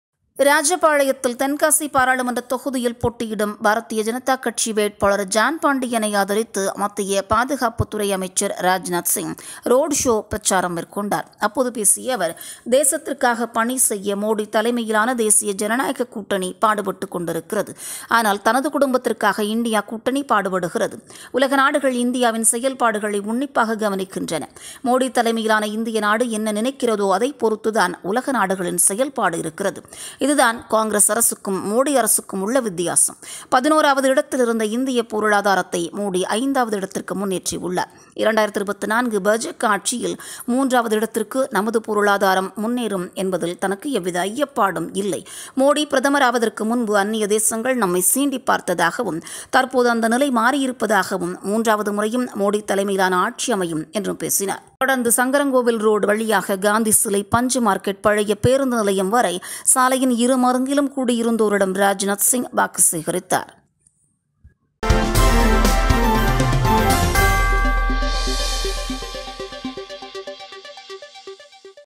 பாரதிய ஜனதா கட்சி மத்திய பாதுகாப்புத்துறை அமைச்சர் ராஜ்நாத் சிங் "ரோடுஷோ"பிரச்சாரம்
மக்களவைத் தேர்தல் 2024

பாரதிய ஜனதா கட்சி மத்திய பாதுகாப்புத்துறை
அமைச்சர் ராஜ்நாத் சிங் "ரோடுஷோ"பிரச்சாரம்  |  Bharatiya Janata Party Central Defense Minister Rajnath Singh's "Roadshow" campaign